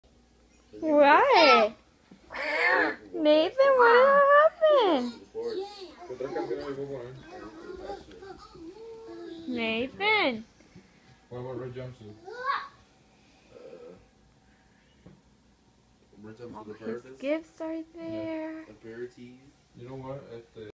Playing with his Christmas gift and birthday
nathan playing with his Christmas gift and birthday!!